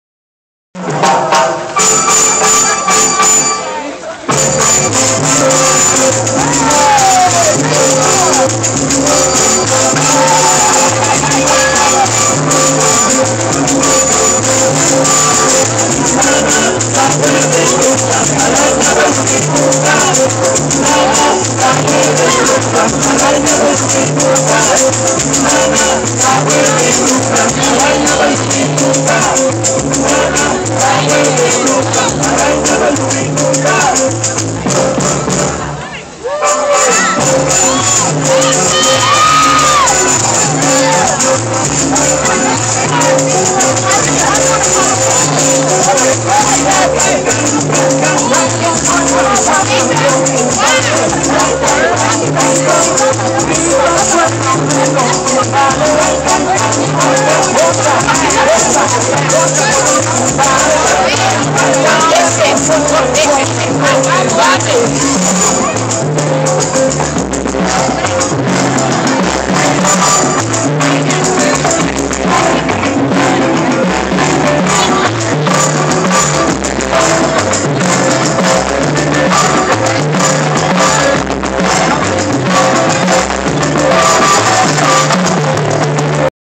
Jennifer Silva - Parkchester Dance Extravaganza
Jennifer Silva performing @ the Parkchester Dance Extravaganza in the Metropolitan Oval in the Bronx.  Jennifer is the Dir. of Siempre en el Ritmo and Bailarinas del Ritmo dance co. in NYC.